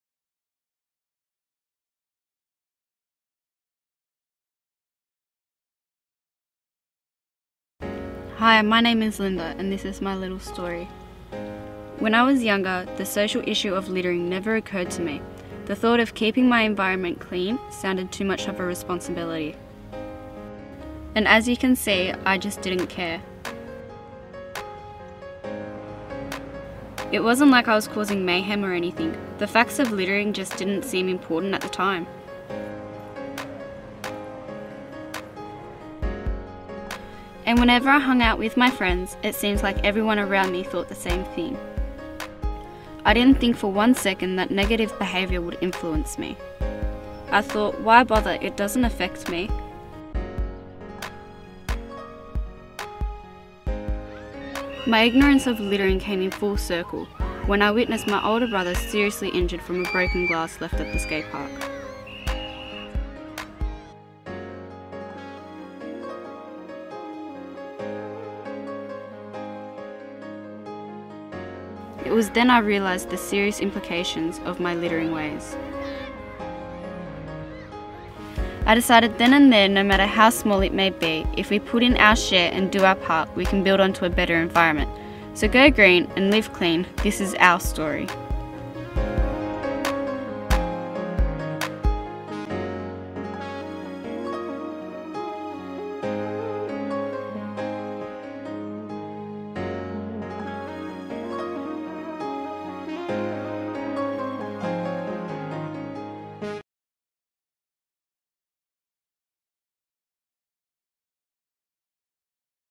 SkateparkLPK
Skatepark@LPK is a short film project created by local young people from Lalor Park (Western Sydney). It focuses on the environmental issues of litter behaviour in areas most frequented by young people (i.e. skate parks and local shops). The short film highlights the positive need to promote litter awareness to our local communities, and the social and environmental consequences of littering. Skatepark@LPK was created during a series of free film workshops hosted by Blacktown City Libraries during National Youth Week 2015.

This project is a NSW EPA Waste Less, Recycle More initiative funded from the waste levy and is proudly supported by Blacktown City Council and Blacktown City Libraries.